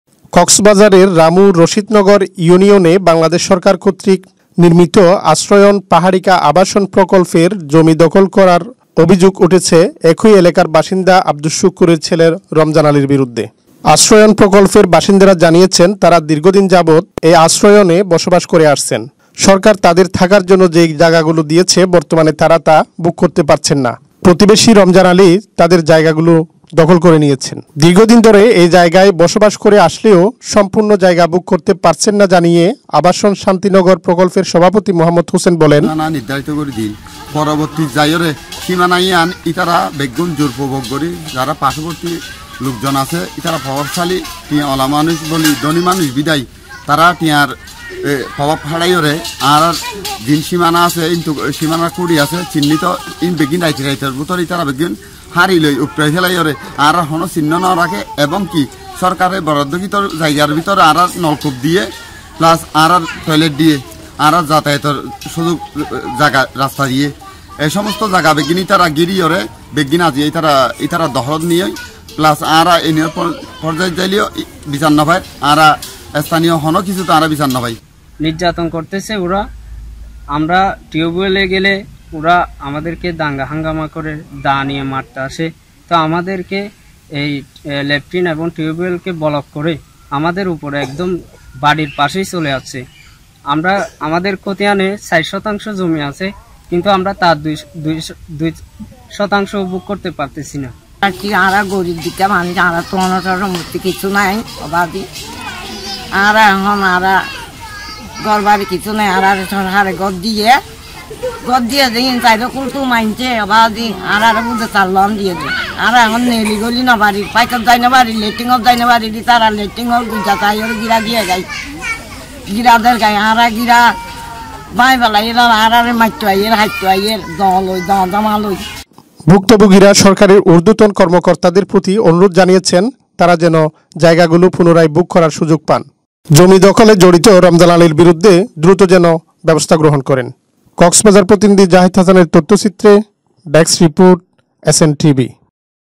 BANGLA NEWS |কক্সবাজার ,রামু রশিদ নগরে আশ্রয়ণ প্রকল্পে জায়গা দখলের অভিযোগ |SN TV 2018
visit:::SNTV.NEWS